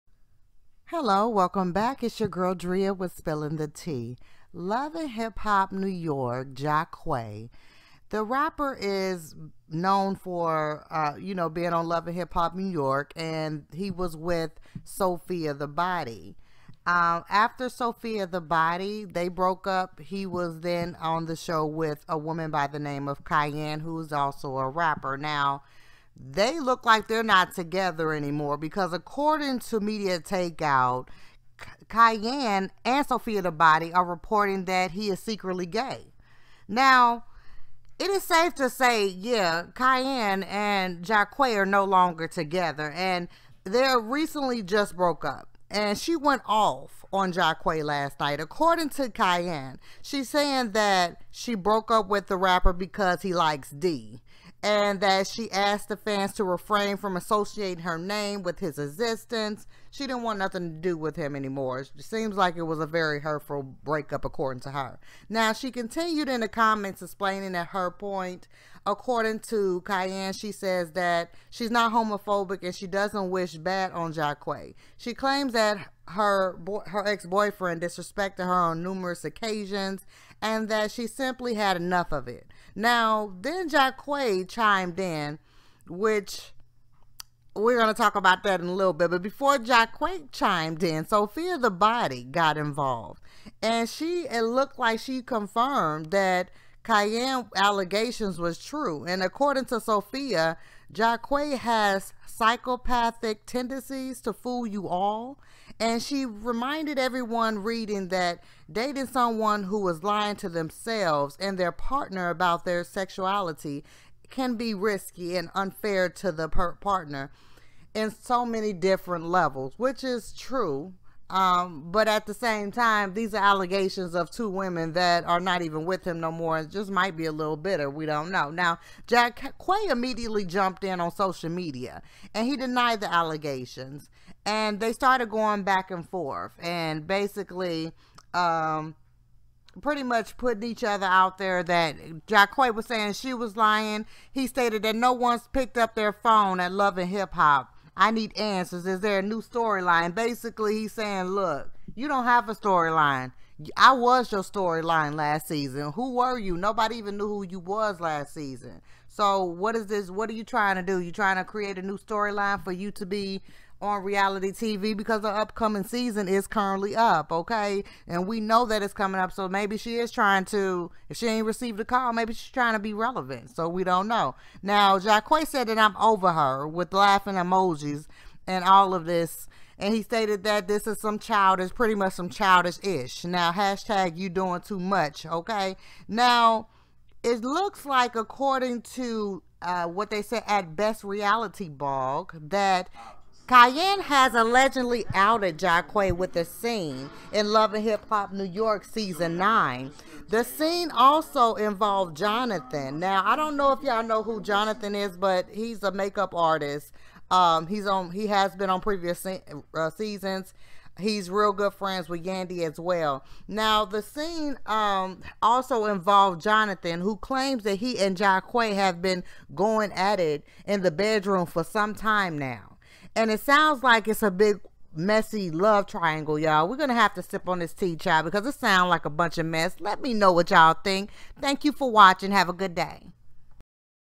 LHHNY Star Allegedly Gay...
According to MTO Jaquae from LHHNY is being accused of being secretly gay, by multiple women. And guess which man they say he is sleeping with on the show.